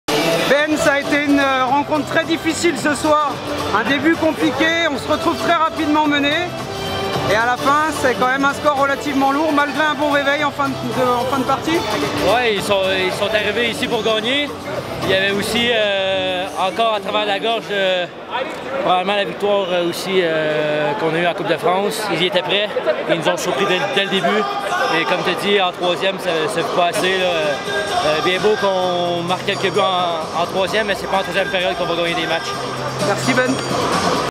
ITW Benjamin Lagarde après Angers
Réaction après la défaite de l'attaquant Pionniers Benjamin Lagarde.